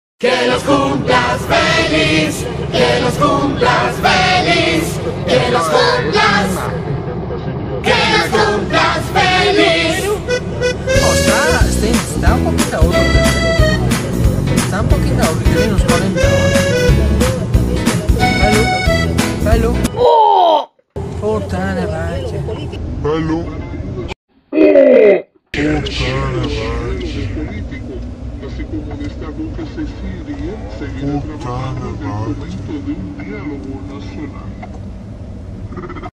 Cerdito se tira del camión para no ir al matadero con música de cumpleaños de fondo [DOBLAJE]
AJJAJAJAAJ gorda putisima cae y esto pasa...